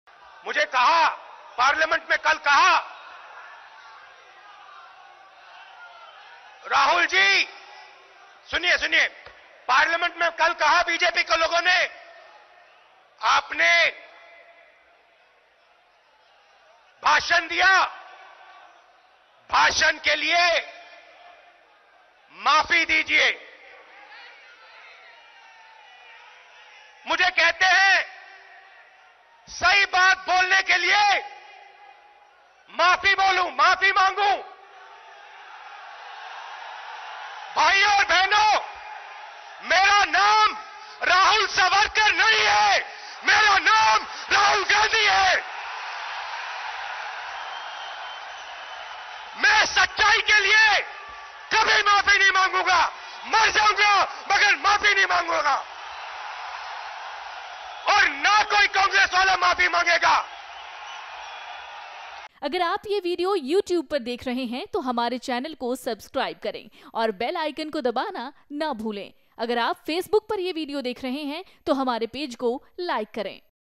Congress Bharat Bachao Rally : राहुल गांधी का जोरदार हमला, 'मेरा नाम Rahul Savarkar नहीं है'
कांग्रेस की 'भारत बचाओ' रैली में राहुल गांधी ने मोदी सरकार पर वार करते हुए कहा कि उनका नाम राहुल सावरकर नहीं है और वो सच्चाई के लिए कभी माफी नहीं मांगेंगे।